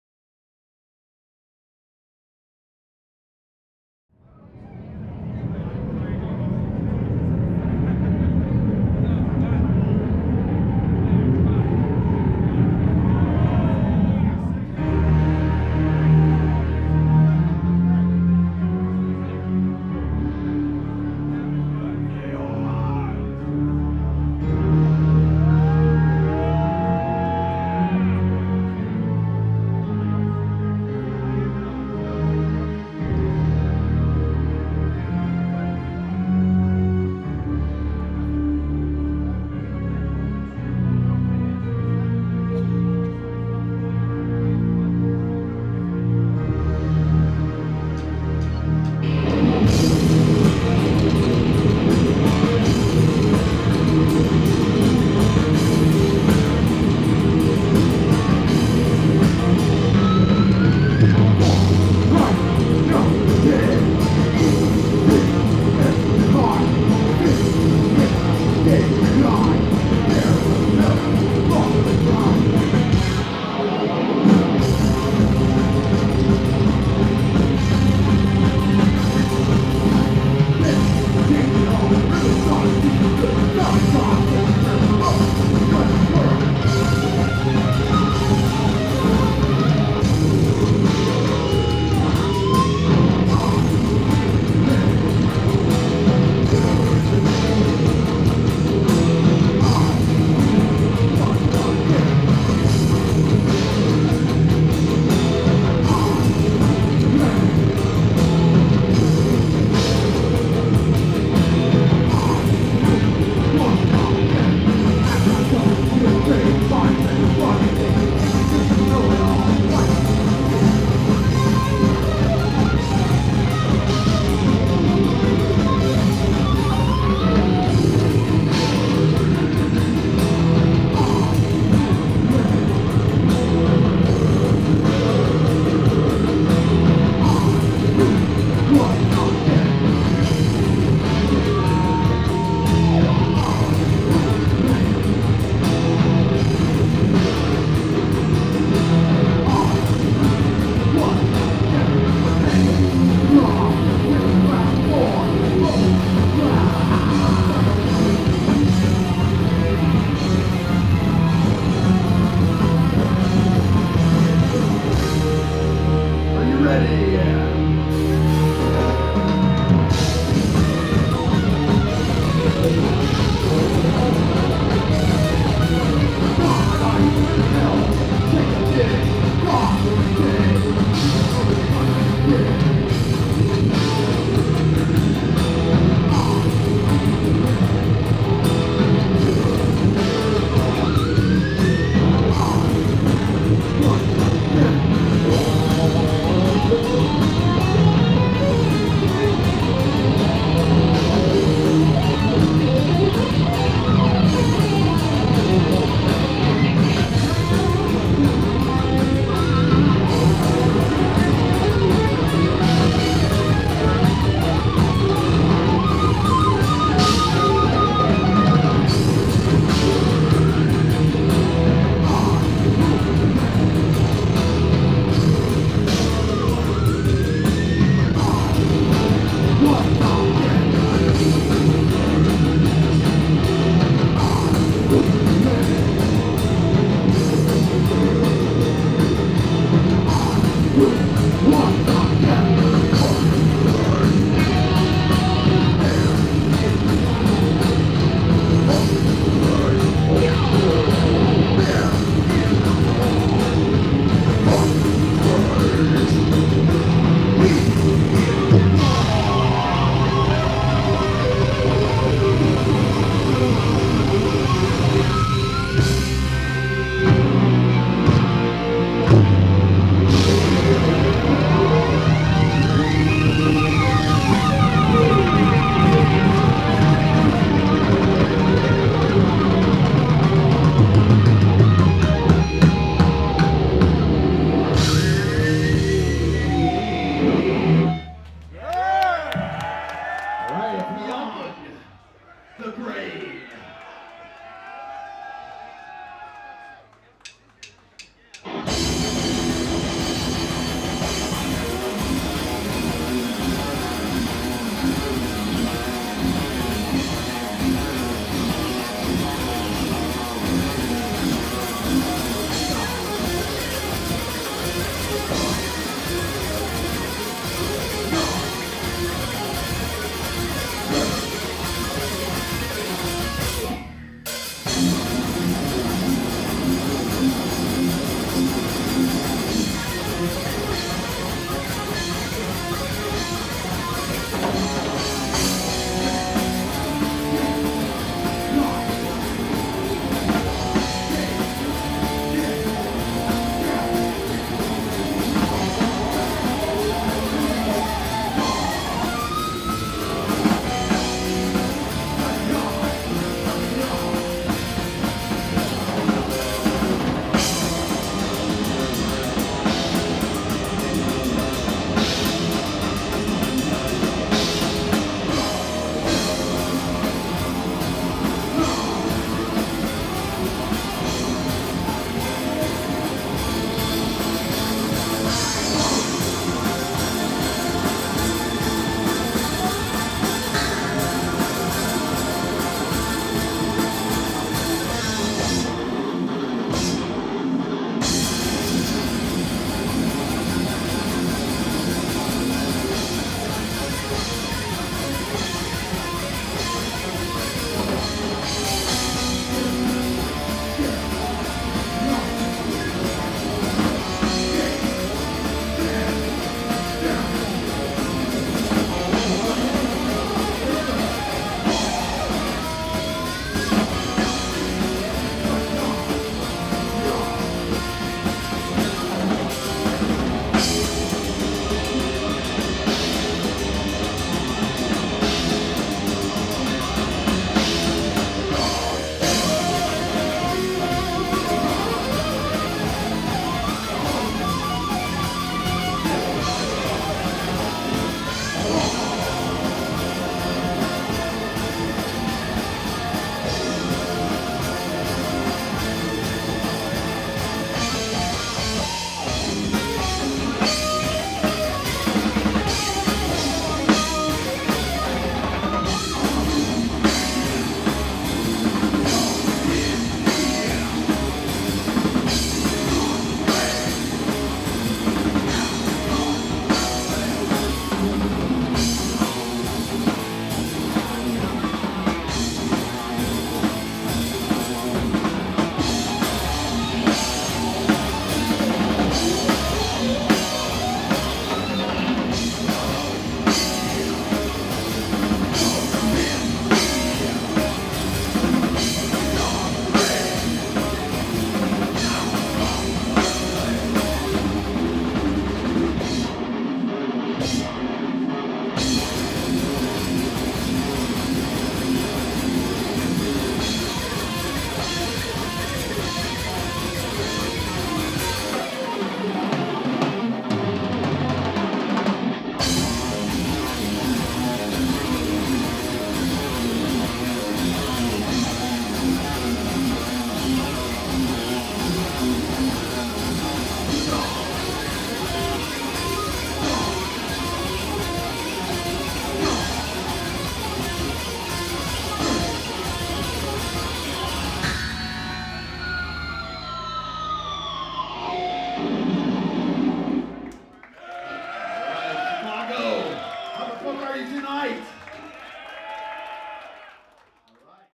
Nocturnus A D live at Reggies 10-3-2015
Nocturnus performing live at Reggies October 3 2015. Murder City Promotions and Deviate & Conquer presentation with supporting acts Blood of The Wolf, Imperial Savagery, Carpathian Funeral and Morgue Supplier.